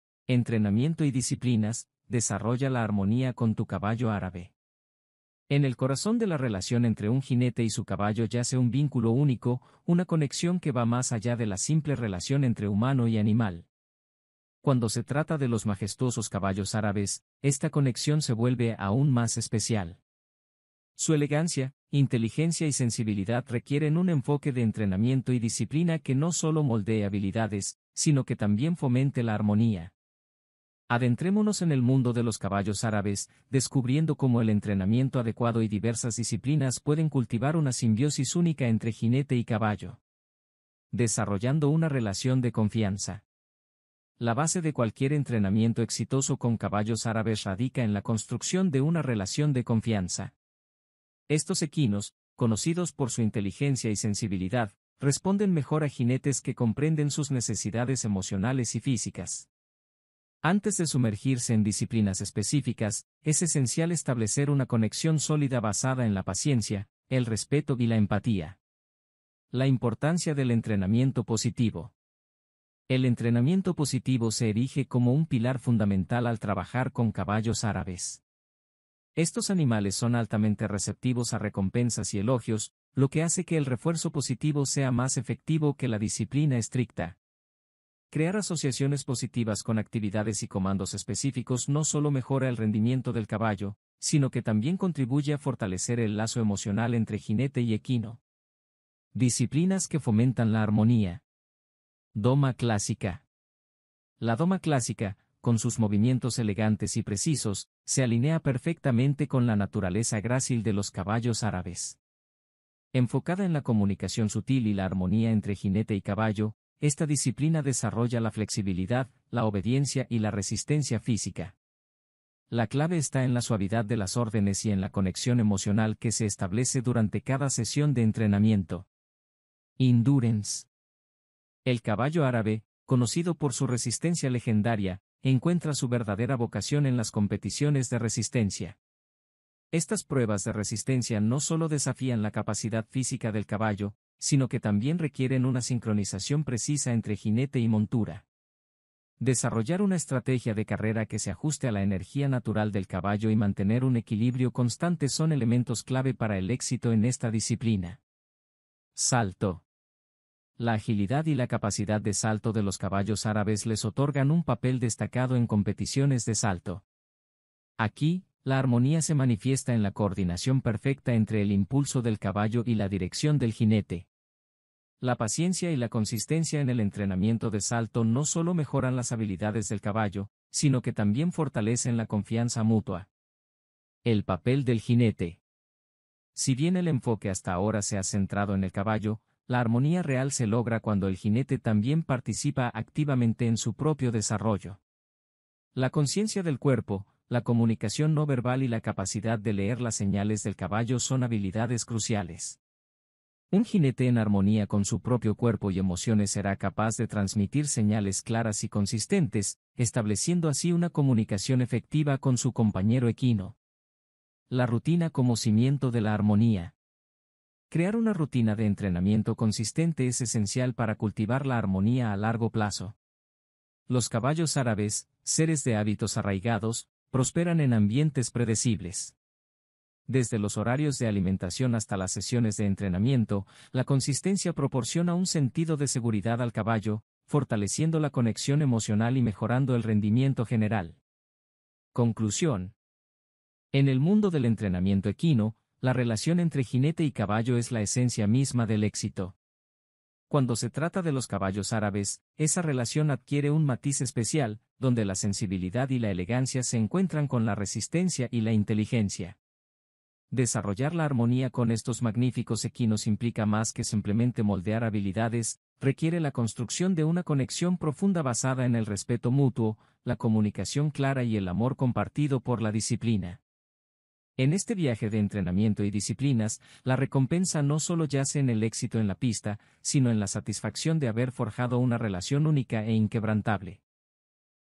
🔴 Entrenamiento y Disciplinas Desarrolla La Armonía Con Tu Caballo ÁRABE 🔴
Entrenamiento y Disciplinas Desarrolla la Armonía con tu Caballo Árabe. En el corazón de la relación entre un jinete y su caballo yace un vínculo único, una conexión que va más allá de la simple relación entre humano y animal. Cuando se trata de los majestuosos Caballos Árabes, esta conexión se vuelve aún más especial. Su elegancia, inteligencia y sensibilidad requieren un enfoque de entrenamiento y disciplina que no solo moldee habilidades, sino que también fomente la armonía. Adentrémonos en el mundo de los Caballos Árabes, descubriendo cómo el entrenamiento adecuado y diversas disciplinas pueden cultivar una simbiosis única entre jinete y caballo.

Desarrollando una Relación de Confianza:

La base de cualquier entrenamiento exitoso con Caballos Árabes radica en la construcción de una relación de confianza. Estos equinos, conocidos por su inteligencia y sensibilidad, responden mejor a jinetes que comprenden sus necesidades emocionales y físicas. Antes de sumergirse en disciplinas específicas, es esencial establecer una conexión sólida basada en la paciencia, el respeto y la empatía.

La Importancia del Entrenamiento Positivo:

El entrenamiento positivo se erige como un pilar fundamental al trabajar con Caballos Árabes. Estos animales son altamente receptivos a recompensas y elogios, lo que hace que el refuerzo positivo sea más efectivo que la disciplina estricta. Crear asociaciones positivas con actividades y comandos específicos no solo mejora el rendimiento del caballo, sino que también contribuye a fortalecer el lazo emocional entre jinete y equino.

Disciplinas que Fomentan la Armonía:

Doma Clásica:
La doma clásica, con sus movimientos elegantes y precisos, se alinea perfectamente con la naturaleza grácil de los Caballos Árabes. Enfocada en la comunicación sutil y la armonía entre jinete y caballo, esta disciplina desarrolla la flexibilidad, la obediencia y la resistencia física. La clave está en la suavidad de las órdenes y en la conexión emocional que se establece durante cada sesión de entrenamiento.

Endurance:
El Caballo Árabe, conocido por su resistencia legendaria, encuentra su verdadera vocación en las competiciones de resistencia. Estas pruebas de resistencia no solo desafían la capacidad física del caballo, sino que también requieren una sincronización precisa entre jinete y montura. Desarrollar una estrategia de carrera que se ajuste a la energía natural del caballo y mantener un equilibrio constante son elementos clave para el éxito en esta disciplina.

Salto:
La agilidad y la capacidad de salto de los Caballos Árabes les otorgan un papel destacado en competiciones de salto. Aquí, la armonía se manifiesta en la coordinación perfecta entre el impulso del caballo y la dirección del jinete. La paciencia y la consistencia en el entrenamiento de salto no solo mejoran las habilidades del caballo, sino que también fortalecen la confianza mutua.

El Papel del Jinete:

Si bien el enfoque hasta ahora se ha centrado en el caballo, la armonía real se logra cuando el jinete también participa activamente en su propio desarrollo. La conciencia del cuerpo, la comunicación no verbal y la capacidad de leer las señales del caballo son habilidades cruciales. Un jinete en armonía con su propio cuerpo y emociones será capaz de transmitir señales claras y consistentes, estableciendo así una comunicación efectiva con su compañero equino.

La Rutina como Cimiento de la Armonía:

Crear una rutina de entrenamiento consistente es esencial para cultivar la armonía a largo plazo. Los Caballos Árabes, seres de hábitos arraigados, prosperan en ambientes predecibles. Desde los horarios de alimentación hasta las sesiones de entrenamiento, la consistencia proporciona un sentido de seguridad al caballo, fortaleciendo la conexión emocional y mejorando el rendimiento general.